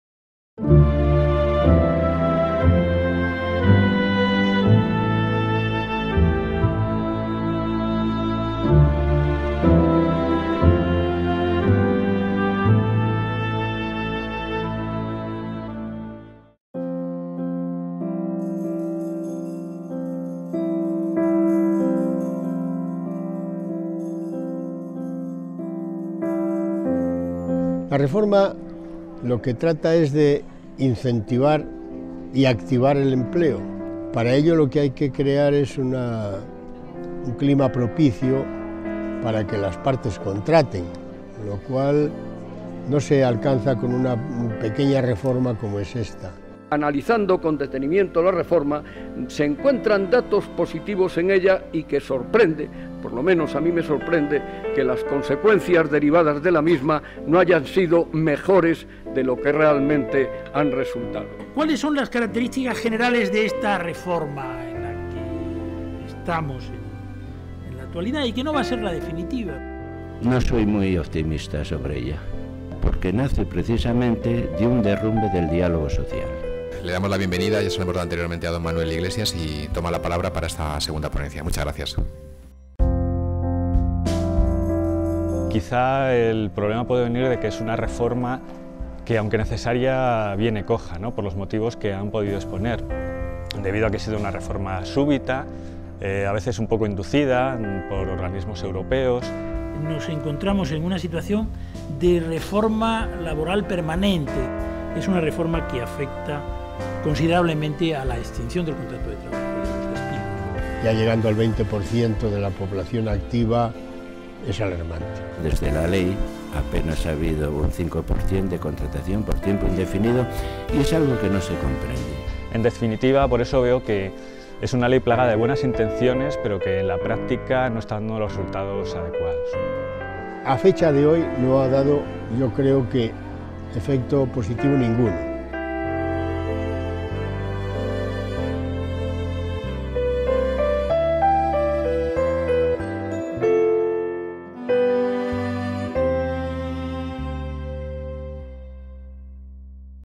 La reforma del mercado laboral
Resumen de las ponencias sobre la Reforma del Mercado Laboral celebradas en el Real Centro Universitario Escorial María Cristina el pasado mes de abril, donde 4 magistrados de la Sala 4ª del Tribunal Supremo, dieron su visión sobre la implantación de la Reforma y los efectos de la misma